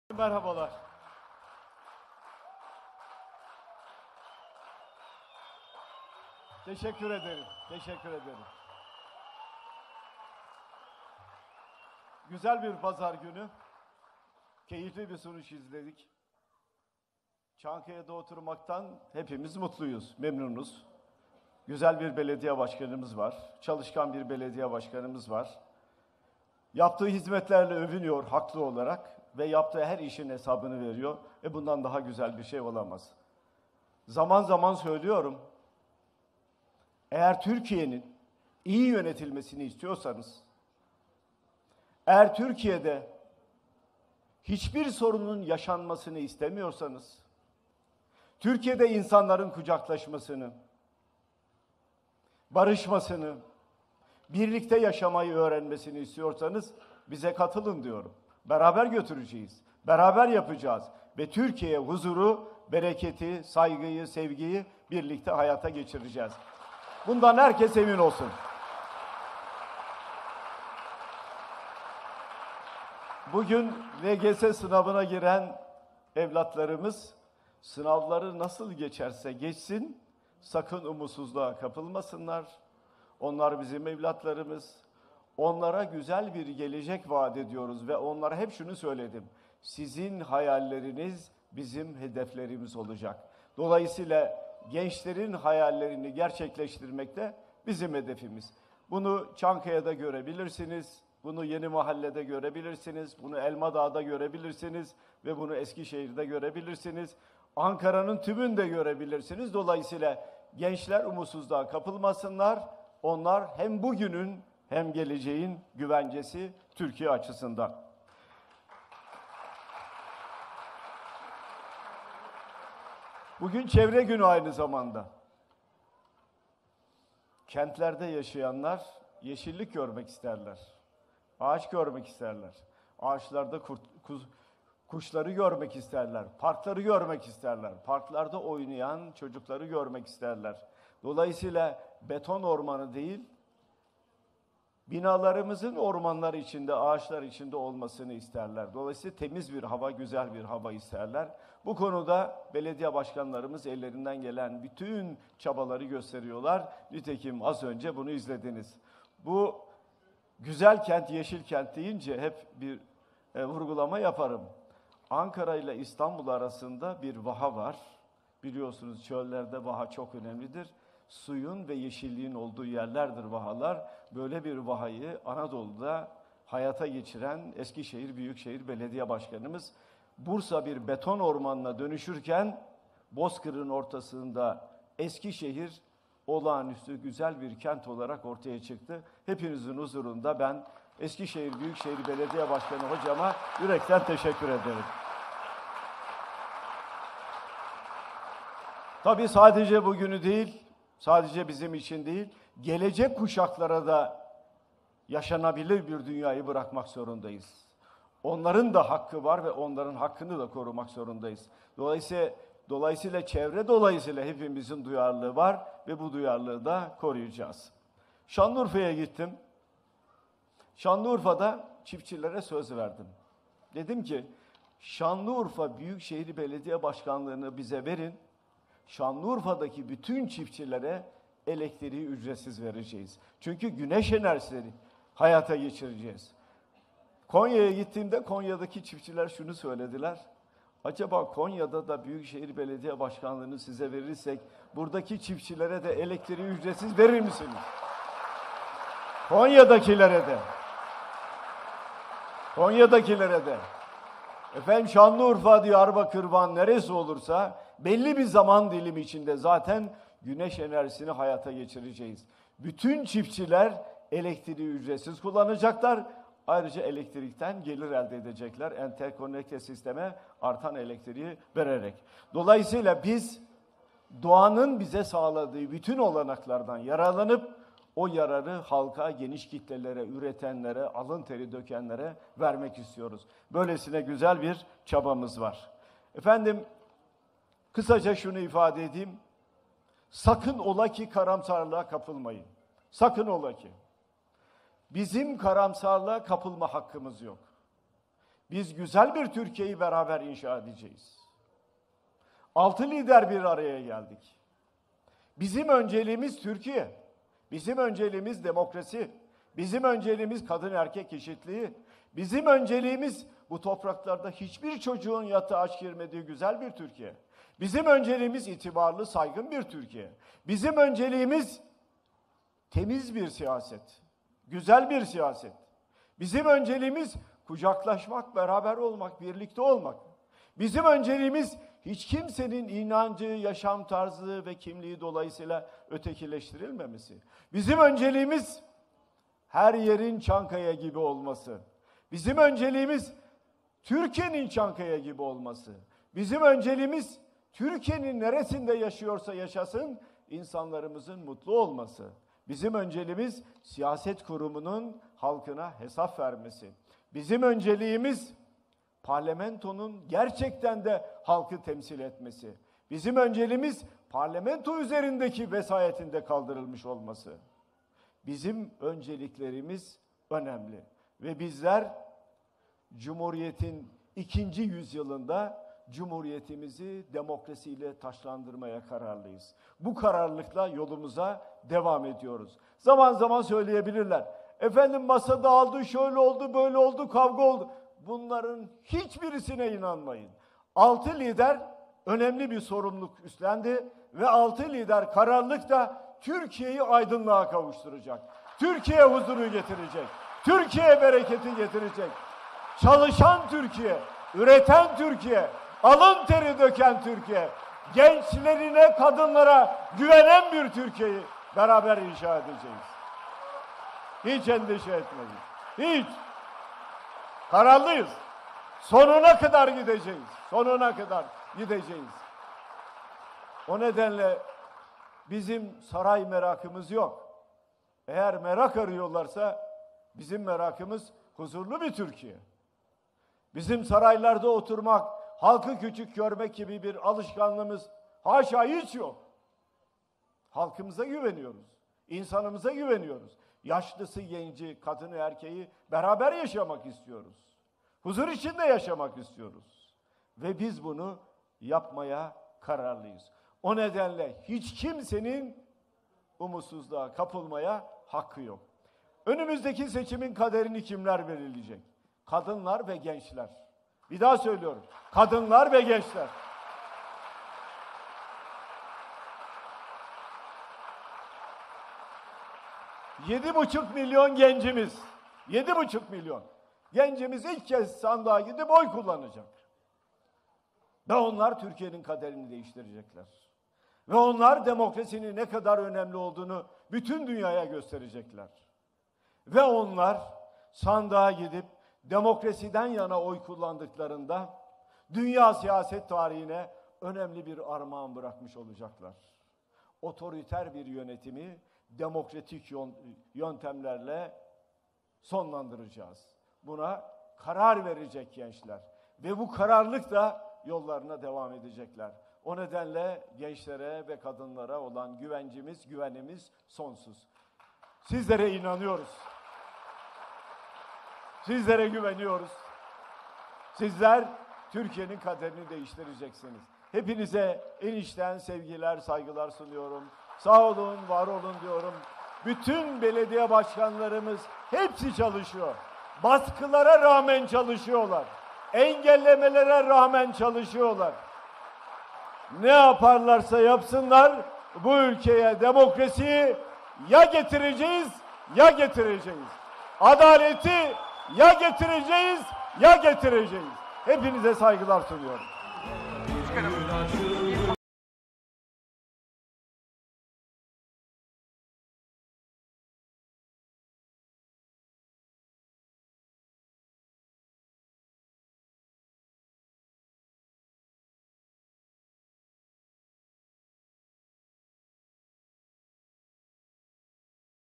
Kemal Kılıçdaroğlu'ndan son dakika 'Altılı Masa' çıkışı: "Önümüzdeki seçimin kaderi..."
Çankaya Belediyesi’nin lansman toplantısında açıklamalarda bulunan CHP lideri Kemal Kılıçdaroğlu, "Altı lider Türkiye’ye huzuru getirecek" dedi.